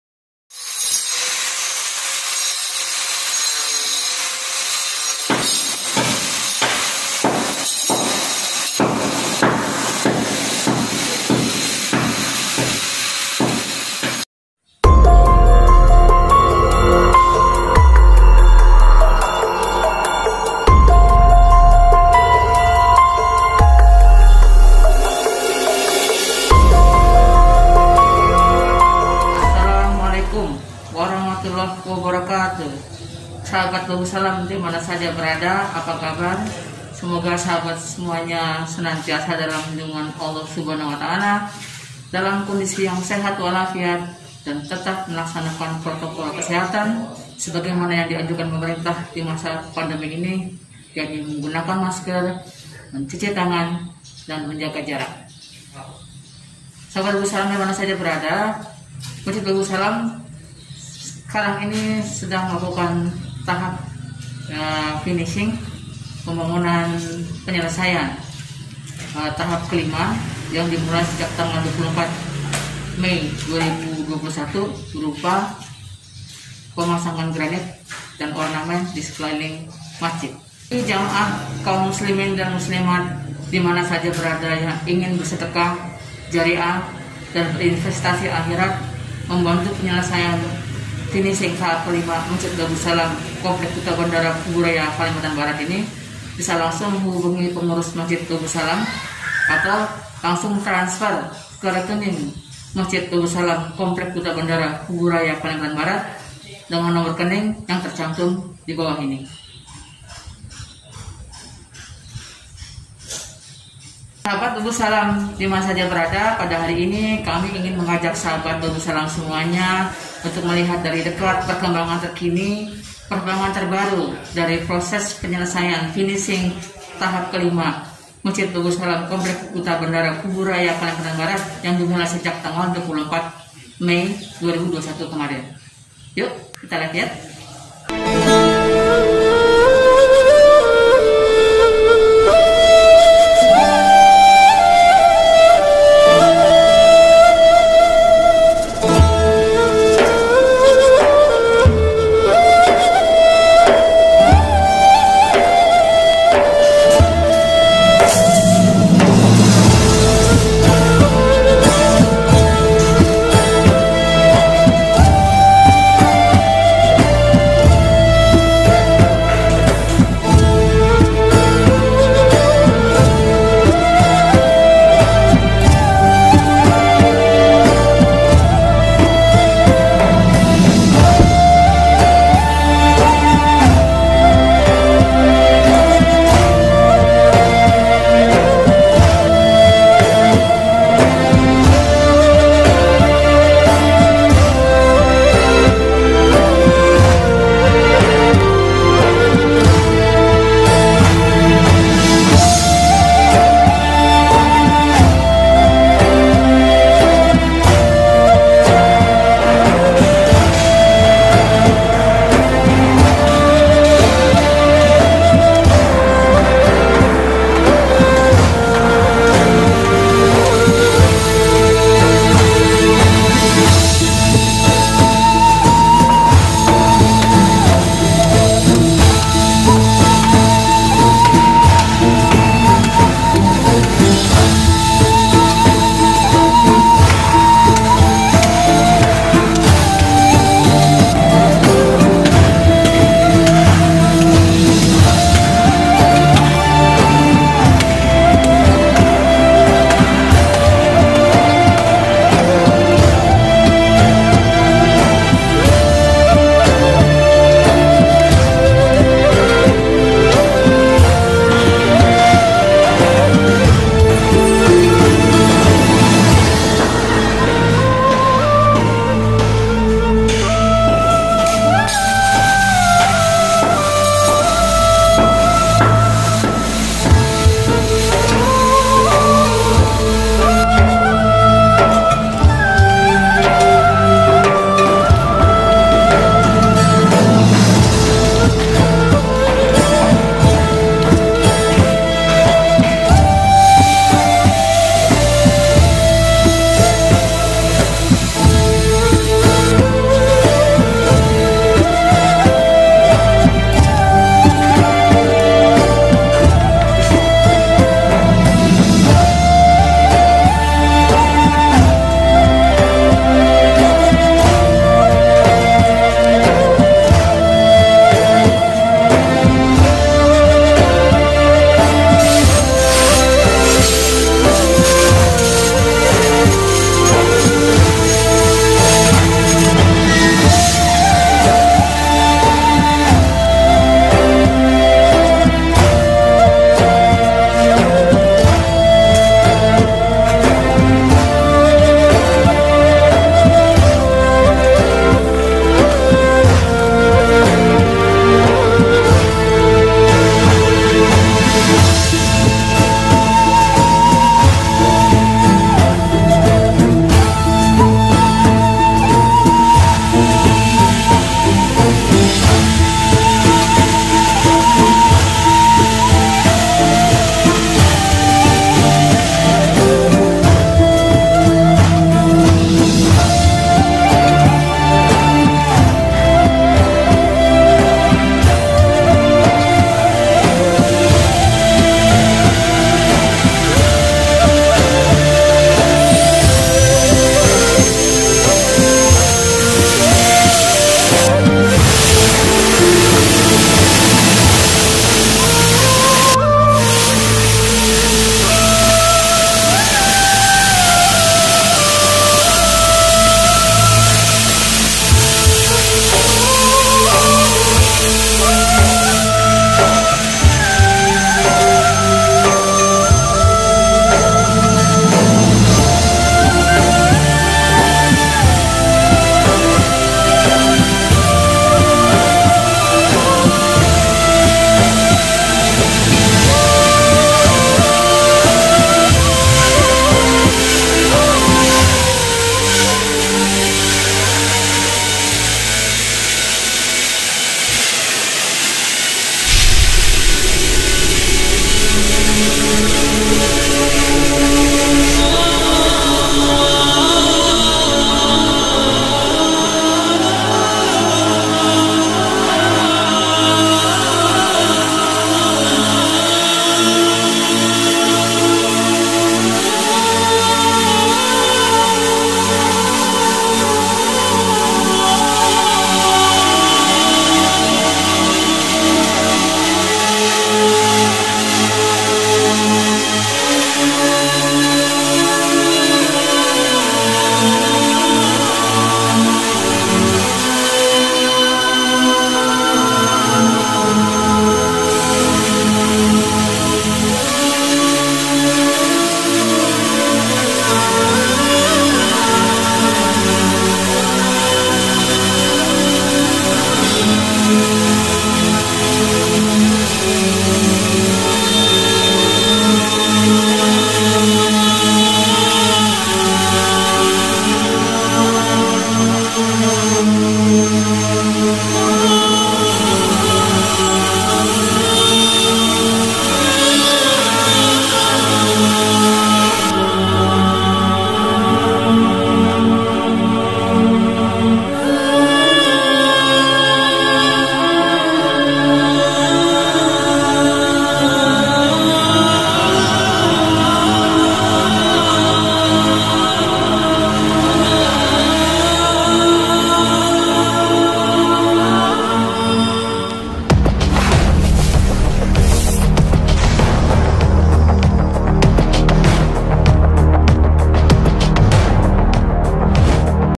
Kabar Terkini Proses Pemasangan Granit Dan Ornamen Dinding Masjid Babussalam Dubai - 2 Juni 2021
Perkembangan terbaru Proses Pemasangan Granit dan Ornamen Masjid Babussalam  Komplek Duta Bandara Kubu Raya -Kalimantan Barat  - 1 Juni 2021

Simak videonya di